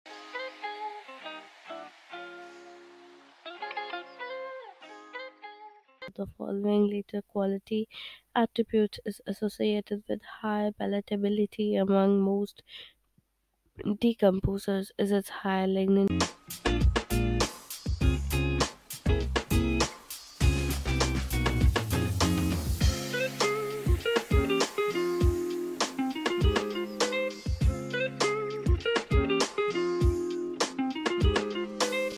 Q4.12. Which of the following litter quality attributes is associated with higher palatability amon…
Q4.12. Which of the following litter quality attributes is associated with higher palatability among most decomposers? High lignin content? High tannin content? High C:N ratio? Low C:N ratio?

Join Numerade today at:
DiscoverthePoweroftheCell:UnleashYourPotentialwithOurProducts